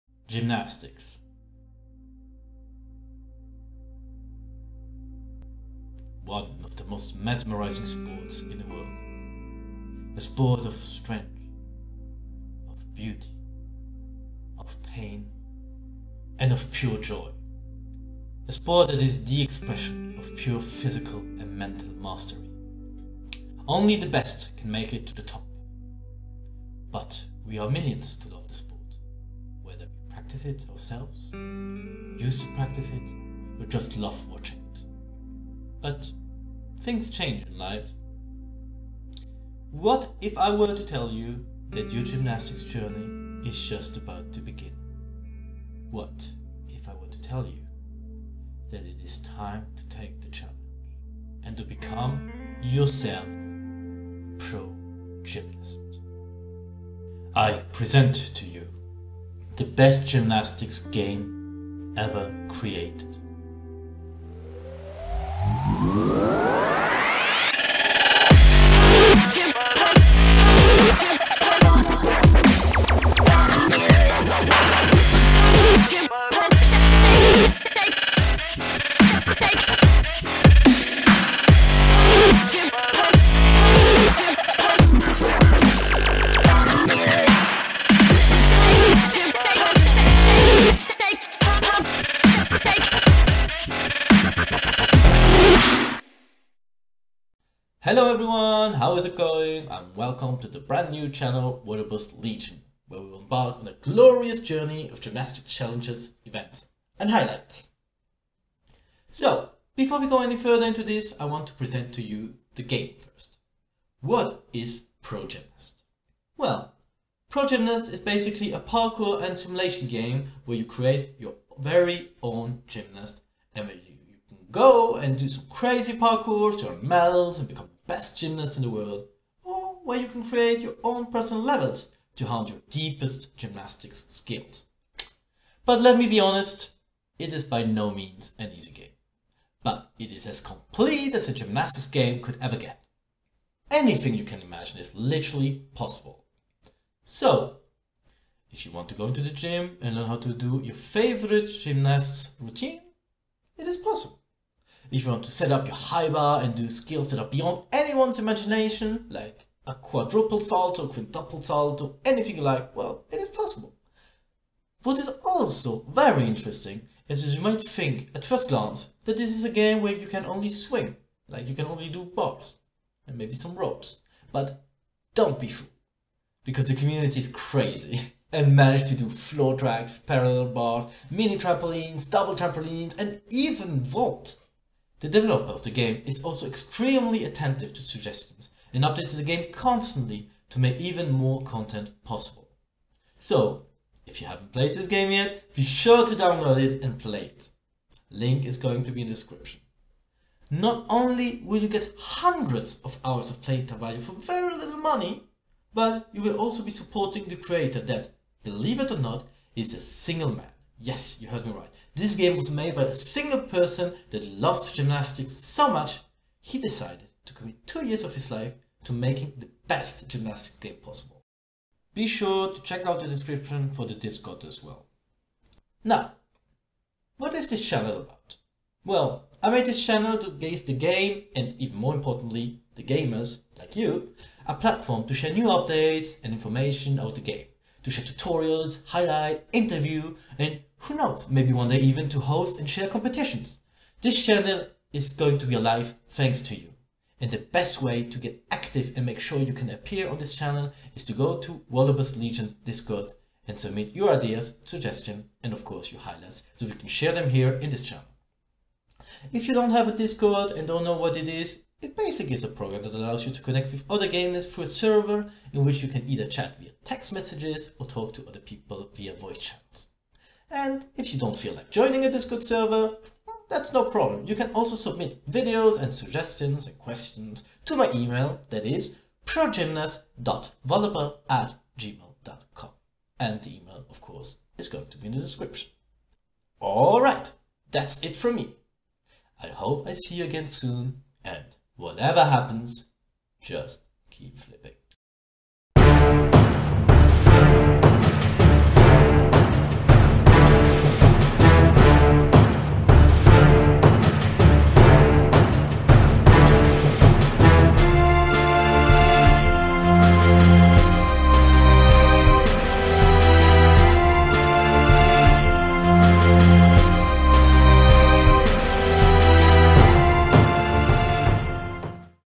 Walaber's Legion - Genesis
0:00 Introduction
1:03 Montage with Game footage
1:37 Channel description
1:49 What is ProGymnast
3:46 Purpose of the Channel